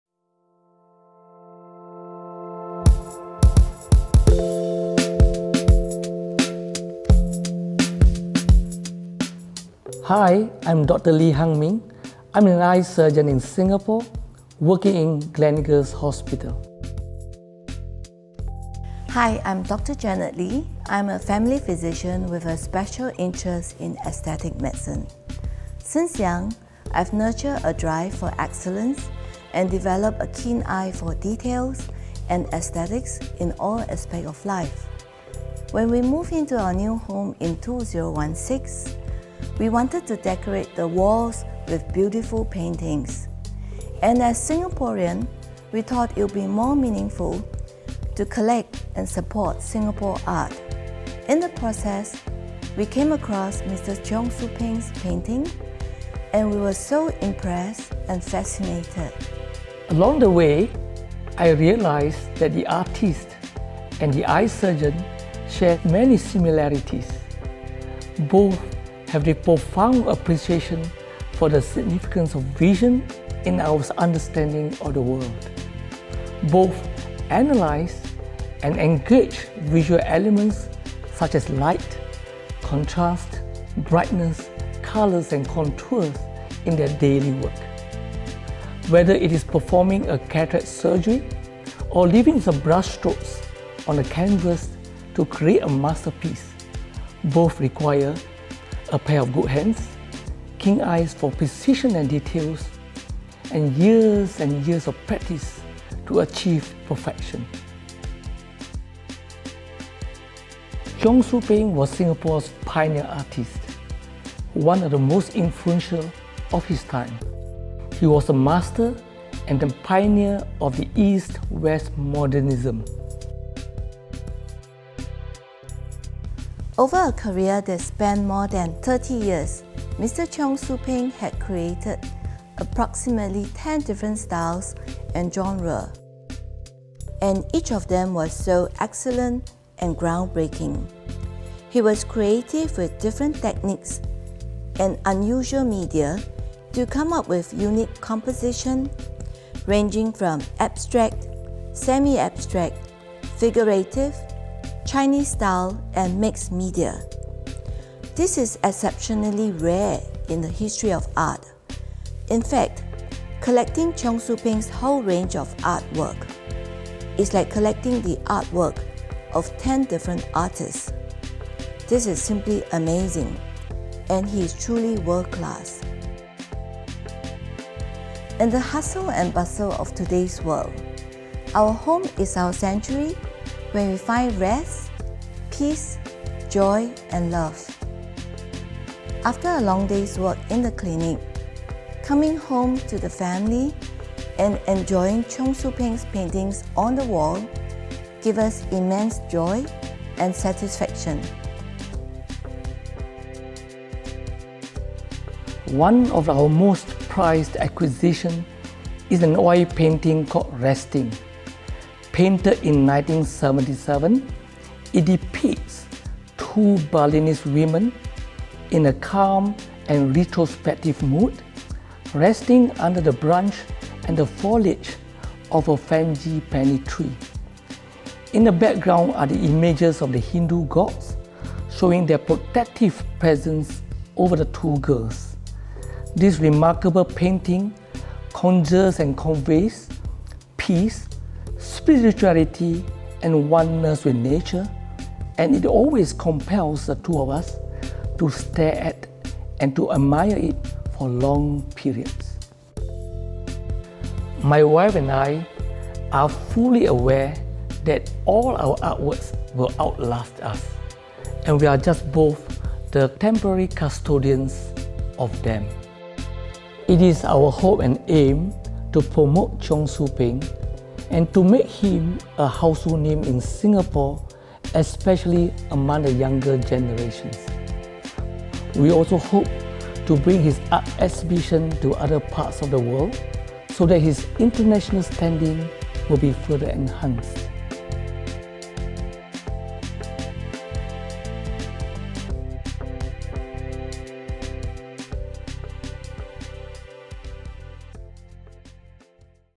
The Art of Cheong Soo Pieng: Inside the Cavern of Dr. Lee Hung Ming and Dr. Janet Lee
16 September - 1 October 2023 |
Cheong Soo Pieng: A Retrospective

Produced in conjunction with the exhibition, Cheong Soo Pieng: A Retrospective, @artcommunegallery6054 is pleased to present 'The Art of Cheong Soo Pieng: Inside the Cavern of Dr. Lee Hung Ming and Dr. Janet Lee.'

Featuring over 100 works of Cheong Soo Pieng, the exhibition will offer a comprehensive look into all aspects of the prolific artist’s career over the span of 40 years. As a pioneer of the Nanyang art style and a renowned figure in the local art scene, his work is widely revered by practitioners and collectors in Singapore and overseas.

This exhibition also marks Cheong’s first retrospective showcase in thirteen years, since his last show at the Singapore Art Museum, and will present an in-depth exploration of the diverse styles and themes that have characterised his career.

The gallery extends our thanks to Jing Wei for the production of this video.